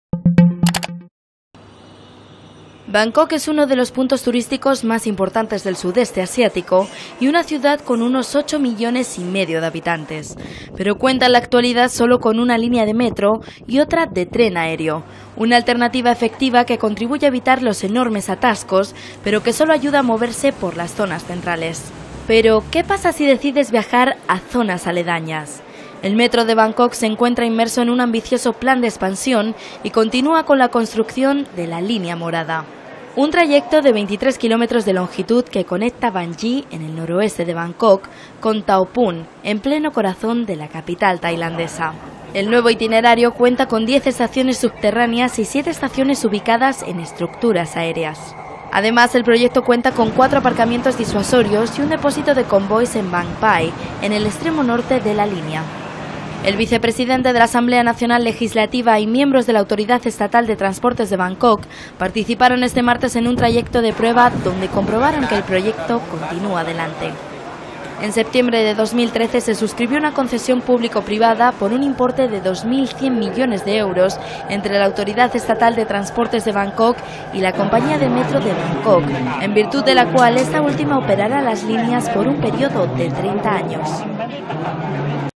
Bangkok expande su metro
Bangkok (Tailandia), 10 may (EFE/EPA).- (Imagen: Narong Sangnak). El metro de Bangkok se encuentra inmerso en un ambicioso plan de expansión, y continúa con la construcción de la Línea Morada, un trayecto de 23 kilómetros de longitud que conecta Bang Yai en el noroeste de la capital con, Tao Pun, en el centro de la ciudad.